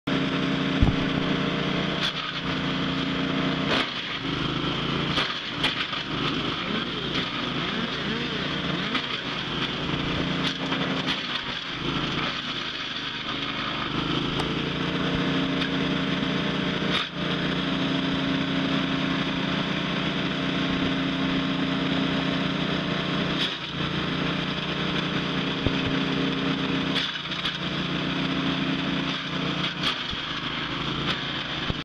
чудо агрегат
пара часов работы и кучи сосновых веток превращаются в сосновые опилки. которые используются для мульчирования